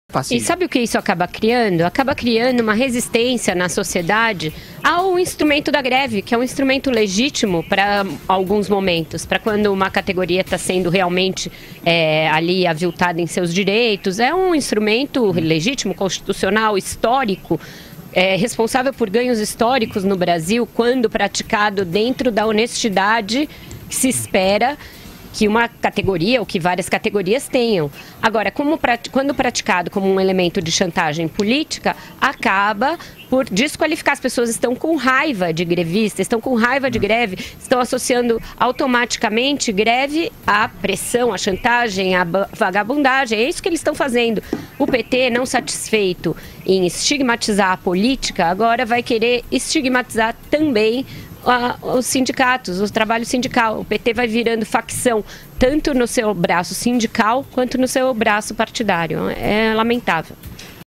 Vera: Além da política, PT estigmatiza também os sindicatos
Greve Geral: Mais categorias aderiram à paralisação convocada para esta sexta-feira por sindicatos contrários às reformas do governo Temer.